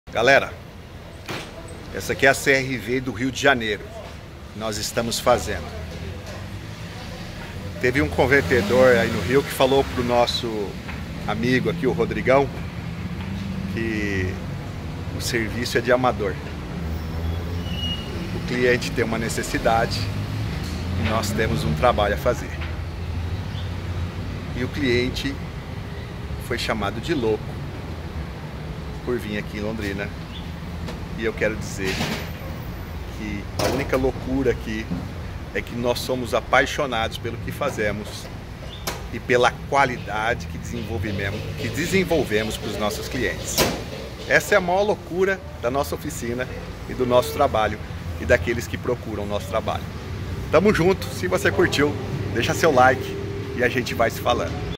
Loucura pela QUALIDADE em Instalação de KIT GNV. CRV TOP NO GÁS
A concorrência não se contem quando encontra alguém sério no mercado de GNV e que AMA O QUE FAZ.
Para aqueles que dizem ser "loucura" trazer o veículo para Londrina, queremos dizer que aqui "somos loucos por QUALIDADE".

tmj